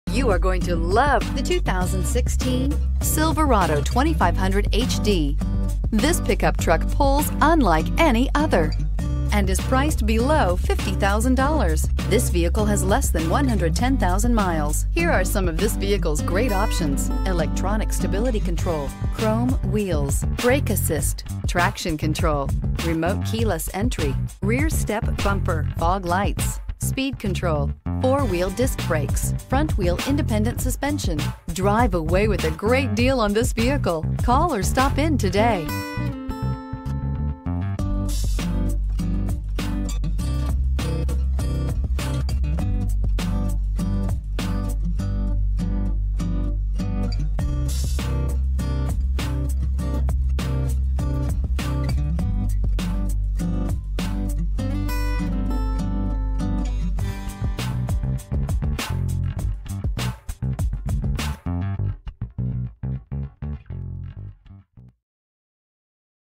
2016 Chevrolet Silverado 2500HD High Country FOR SALE in Tomball, TX 185269N
Stop in today for a test drive of this U 2016 Chevrolet Silverado 2500HD

Parkway Chevrolet
25500 State Highway 249

Black 2016 Chevrolet Silverado 2500HD High Country 4WD Allison 1000 6-Speed Automatic Duramax 6.6L V8 Turbodiesel  Recent Arrival!   Free car washes for 6 months with Purchase of Vehicle!  Reviews:     Impressive hauling and towing capabilities; refined and quiet ride; strong diesel engine; available CNG fueling for regular V8; solid build quality. Source: Edmunds